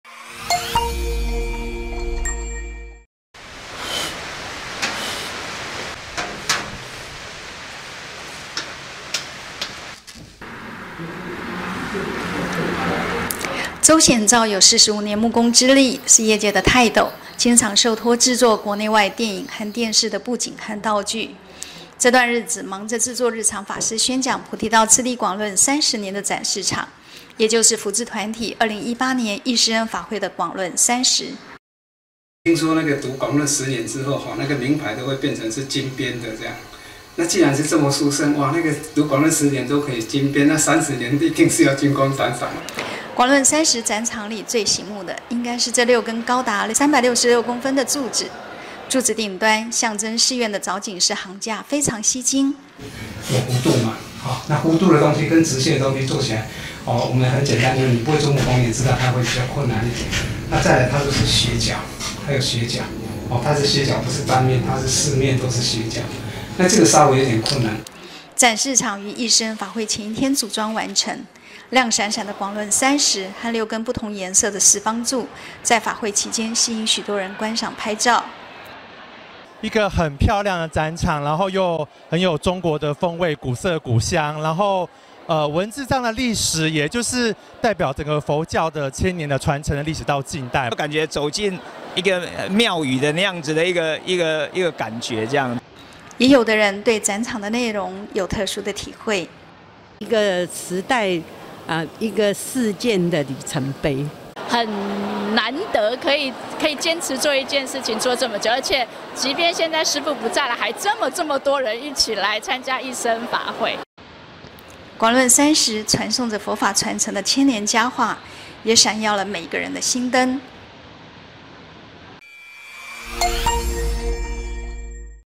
《 廣論30展場・宛若寺院 》
福智團體憶師恩法會上，今年適逢創辦人日常老和尚宣講廣論30的時間，在法會展場上，就特別規劃〈廣論30〉的主題。這個展場是如何規畫製作的呢？帶您回到法會開始的前幾天，來看這展場設計佈置的幕後人員。